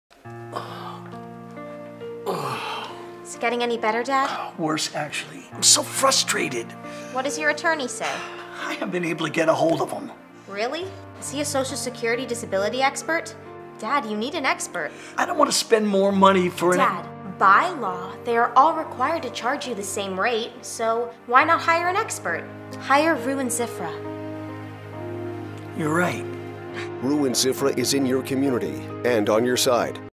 Rue & Ziffra - "SSD Father-Daughter
Video production by Blue Dog Video. :30 Social Security-Disability commercial for Rue & Ziffra.